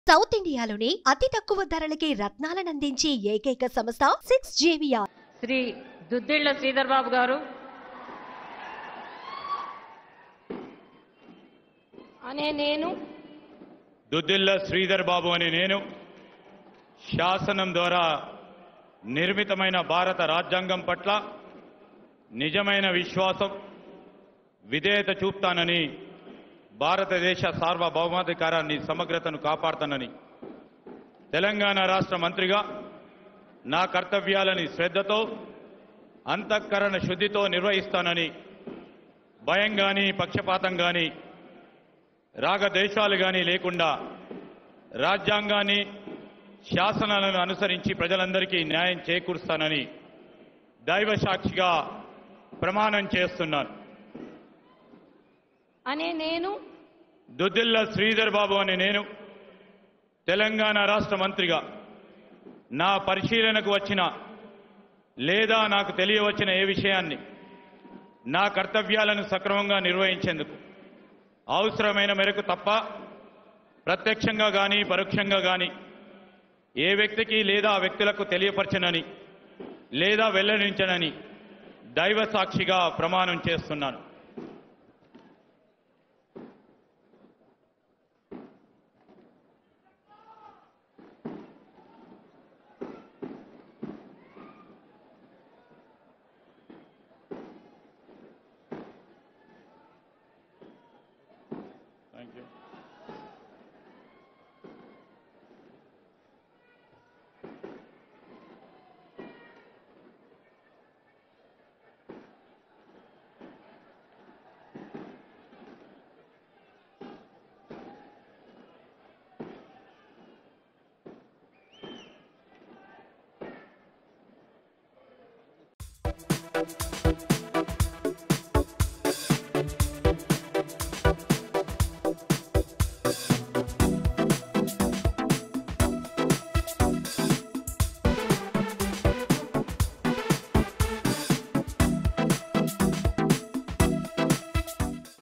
మంత్రిగా దుద్దిళ్ల శ్రీధర్ బాబు ప్రమాణ స్వీకారం | Minister Duddilla Sridhar Babu
Watch►మంత్రిగా దుద్దిళ్ల శ్రీధర్ బాబు ప్రమాణ స్వీకారం | Minister Duddilla Sridhar Babu |@sumantvdharmapuri